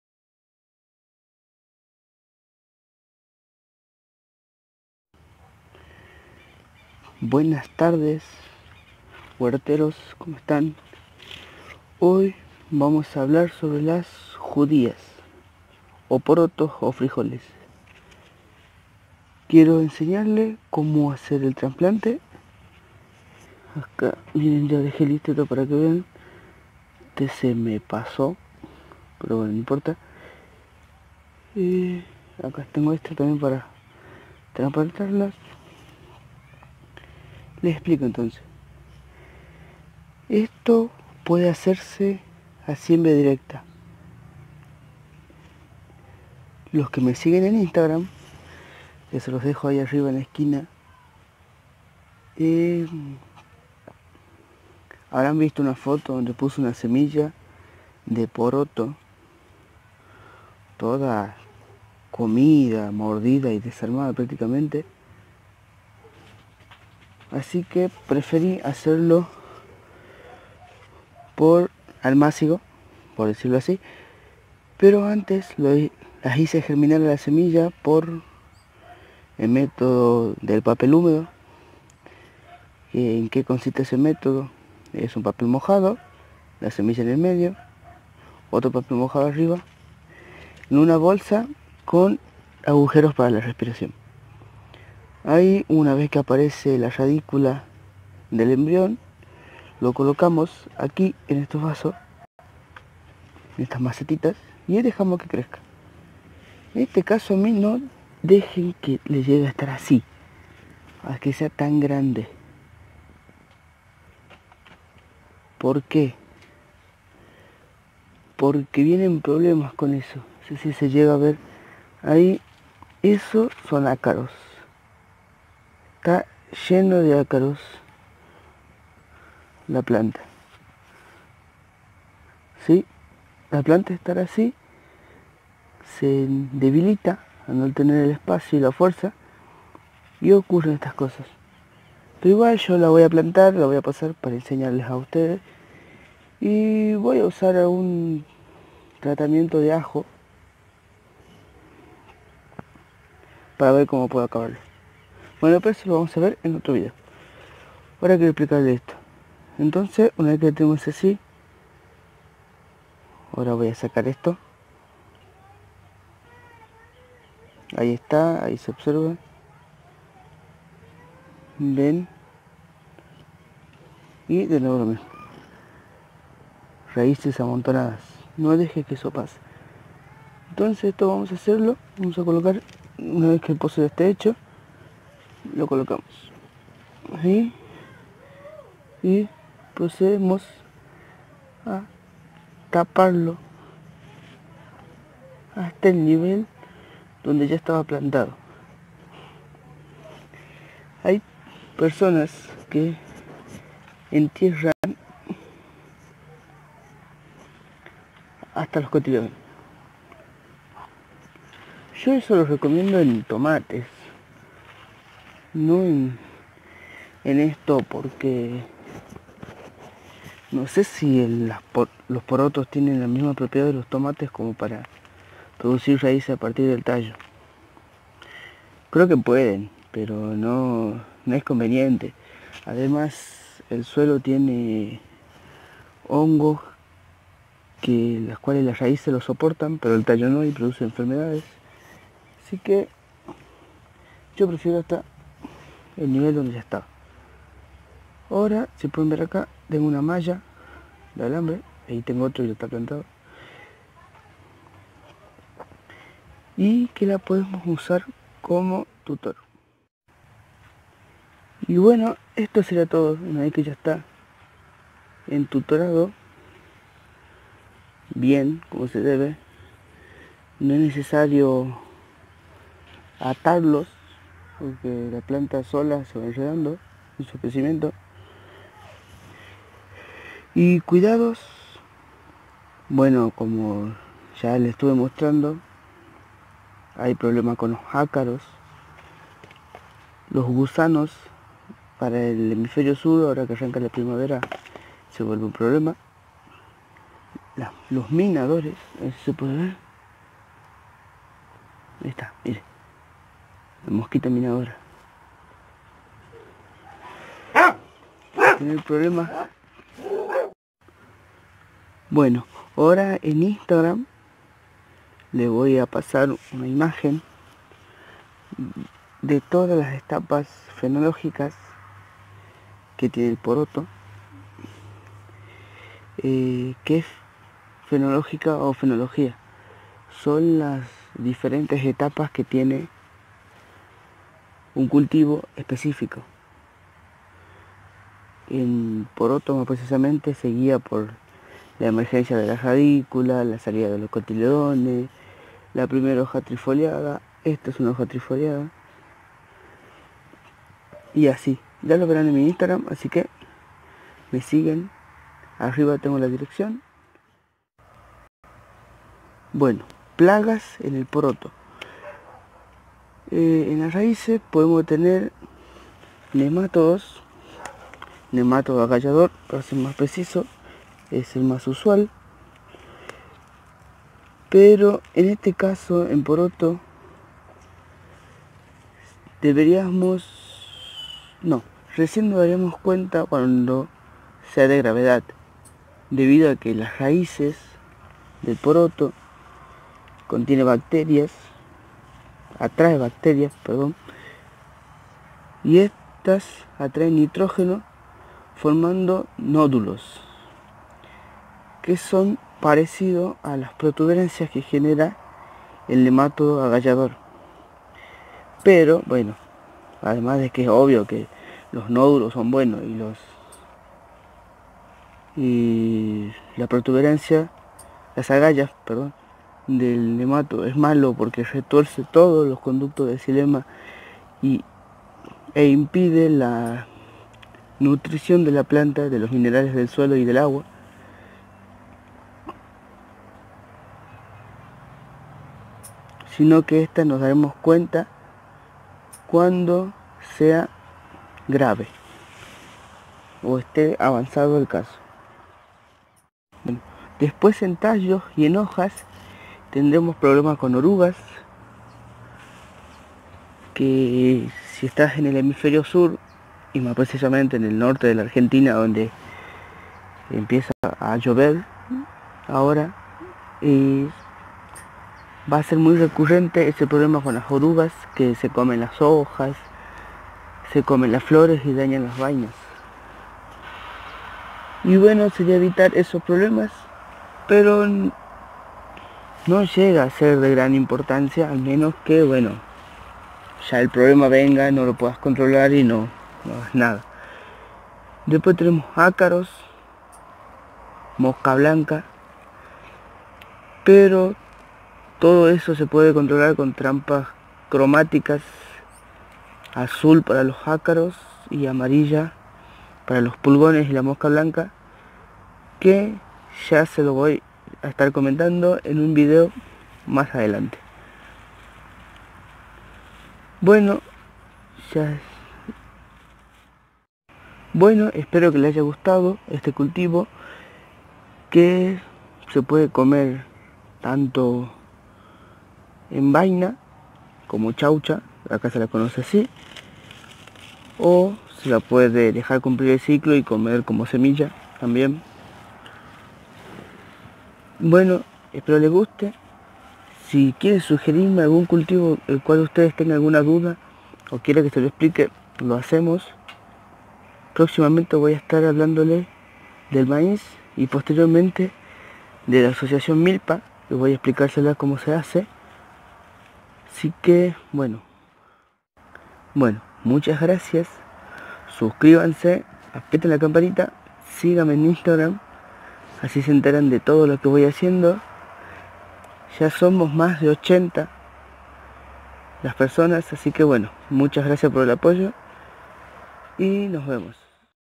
Como sembrar JUDIAS/FRIJOLES/POROTOS !!!!!!||COMO CULTIVAR||👏👏👏 1era parte de la Asociación MILPA
Como sembrar porotos /judias en el huerto de una manera sencilla y rápida,  recordar que es una leguminosas de primavera-verano con el beneficio de captación de nitrógeno en el suelo.